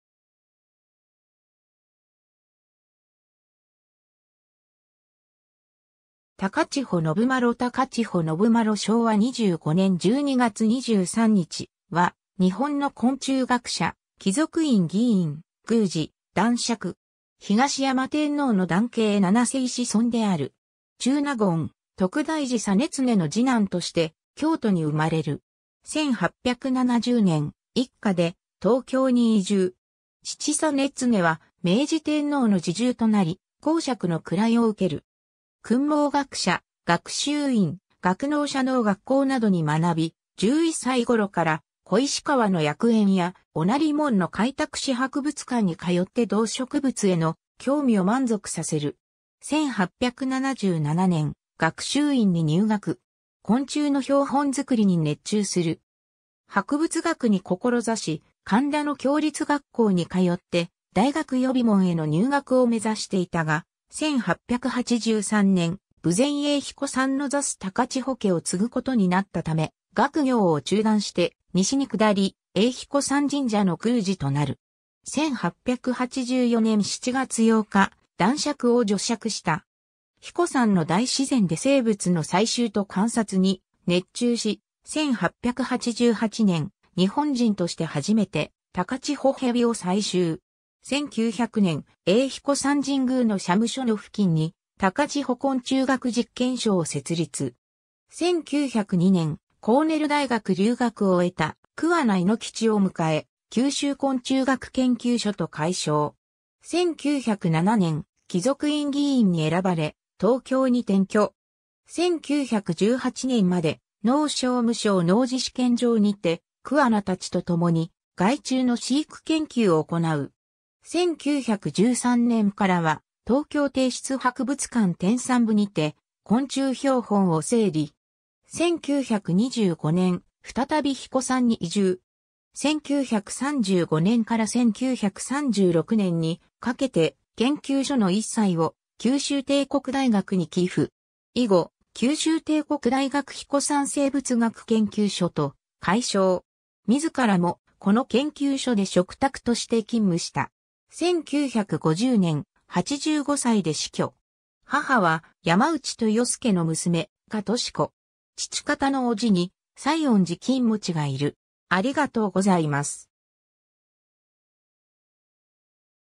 高千穂宣麿
高千穂宣麿 高千穂 宣麿（たかちほ のぶまろ、元治元年12月15日（1865年1月12日） - 昭和25年（1950年）12月23日）は日本の昆虫学者、貴族院議員、宮司、男爵。
東山天皇の男系七世子孫である。
 中納言徳大寺実則の次男として京都に生まれる。
1870年、一家で東京に移住。
父実則は明治天皇の侍従となり、侯爵（のち公爵）の位を受ける。
 訓蒙学舎、学習院、学農社農学校などに学び、11歳頃から小石川の薬園（東京大学小石川植物園）や御成門の開拓使博物館に通って動植物への興味を満足させる。
 1877年、学習院に入学。
昆虫の標本作りに熱中する。
博物学に志し、神田の共立学校（開成中学校・高等学校）に通って、大学予備門への入学を目指していたが、1883年、豊前英彦山の座主高千穂家を継ぐことになったため、学業を中断して西に下り、英彦山神社の宮司となる。
1884年（明治17年）7月8日、男爵を叙爵した。
 英彦山の大自然で生物の採集と観察に熱中し、1888年、日本人として初めてタカチホヘビを採集。
 1900年、英彦山神宮の社務所の付近に高千穂昆虫学実験所を設立。
1902年、コーネル大学留学を卒えた桑名伊之吉を迎え、九州昆虫学研究所と改称。
 1907年、貴族院議員に選ばれ東京に転居。
1918年まで、農商務省農事試験場にて、桑名たちと共に害虫の飼育研究をおこなう。
1913年からは東京帝室博物館（東京国立博物館）天産部にて昆虫標本を整理。
1925年、再び彦山に移住。
 1935年から1936年にかけて、研究所の一切を九州帝国大学に寄付。
以後、九州帝国大学彦山生物学研究所と改称。
自らもこの研究所で嘱託として勤務した。
1950年、85歳で死去。
 母は山内豊資の娘・嘉年子。
父方の叔父に西園寺公望がいる。